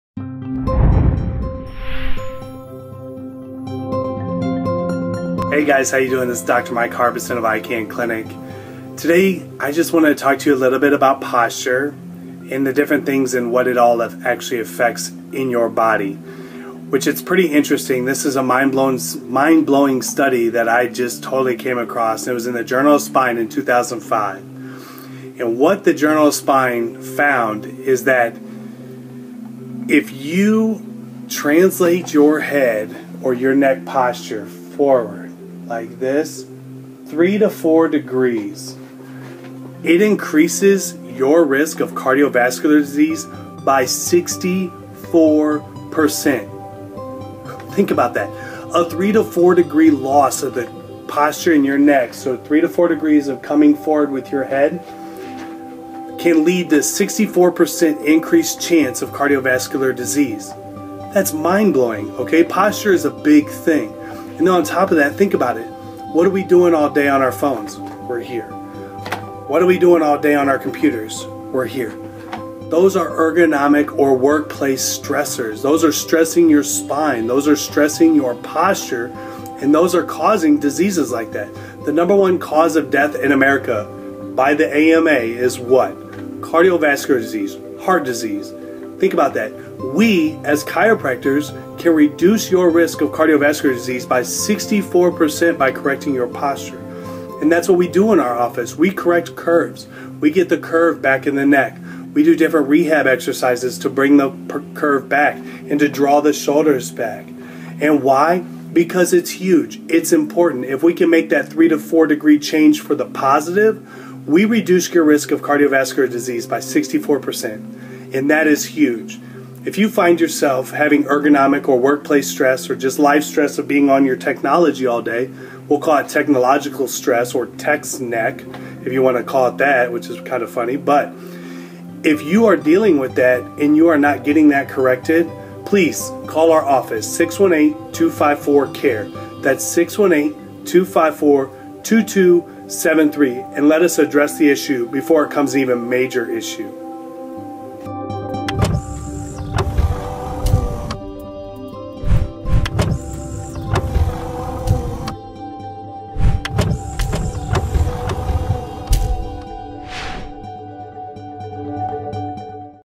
Dr. Mike Harbison of ICAN Clinic Discussing Posture
Dr. Mike Harbison of ICAN Clinic discussing how poor posture alters the status of your health.